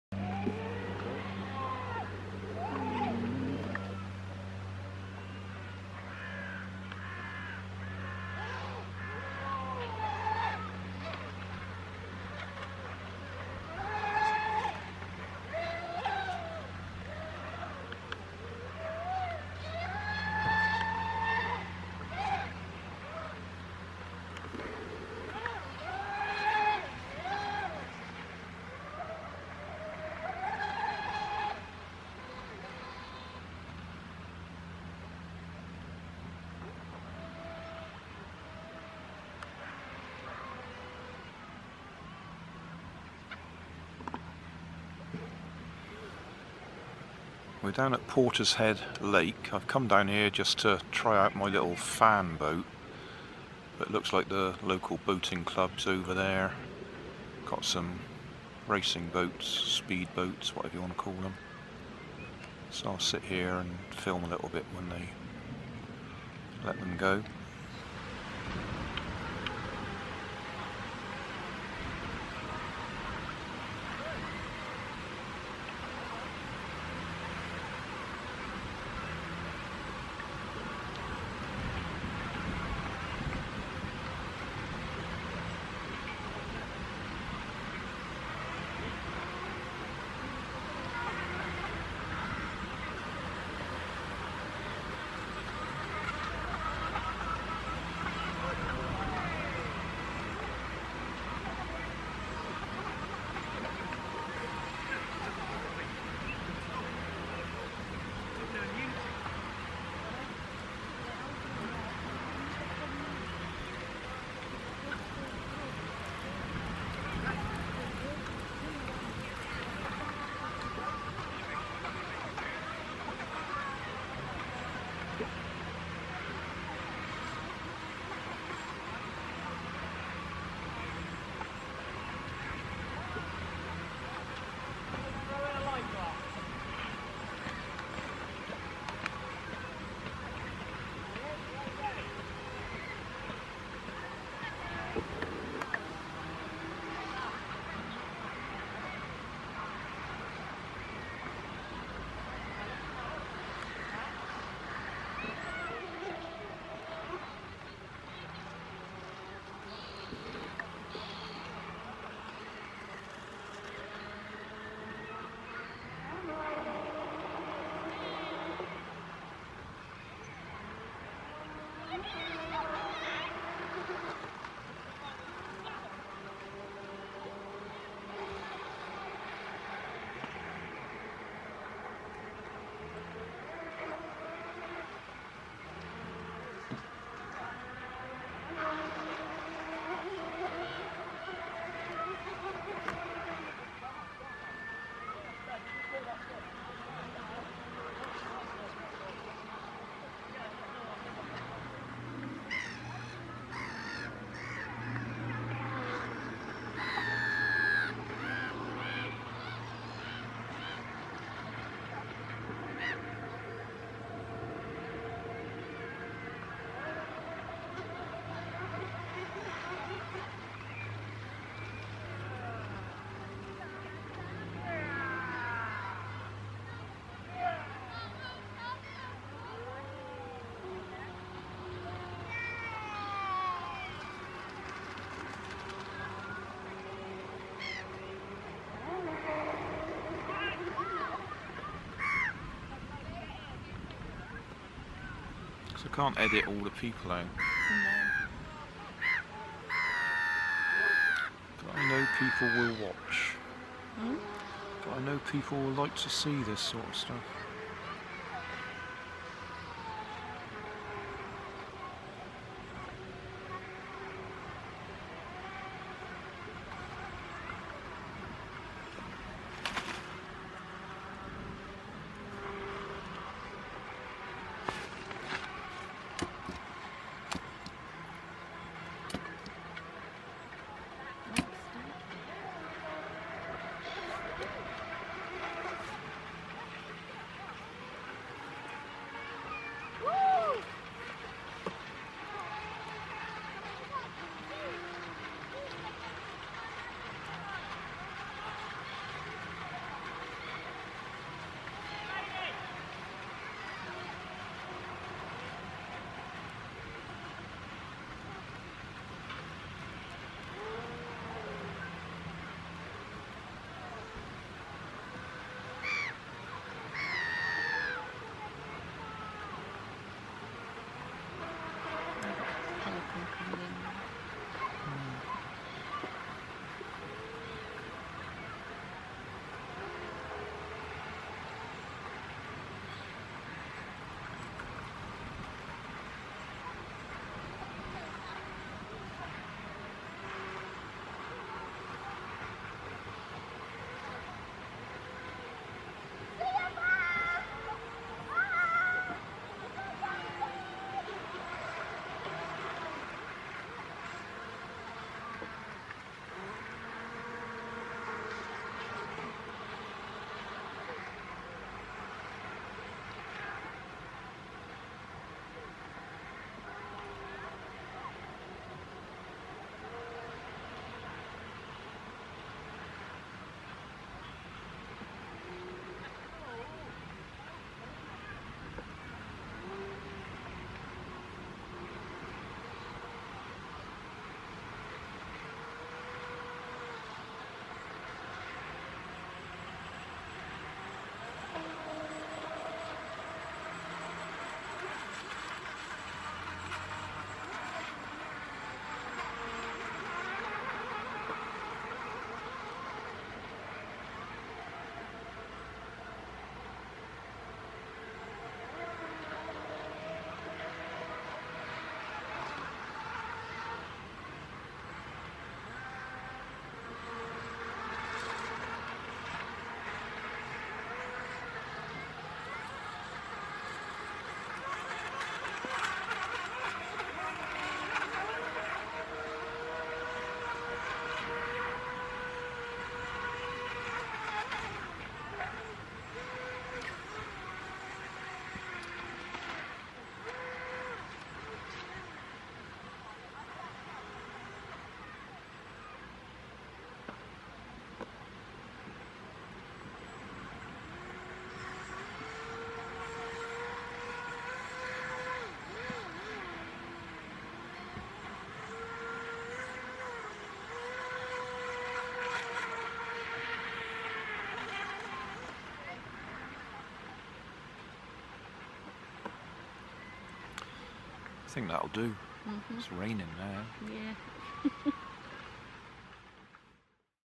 Fast RC boats on Portishead lake
Fast RC boats on Portishead lake. Came down here to try my quad fan airboat that I have done a little modification to. It was really too windy for it so I decided to film a few of the fast RC boats that were on the water.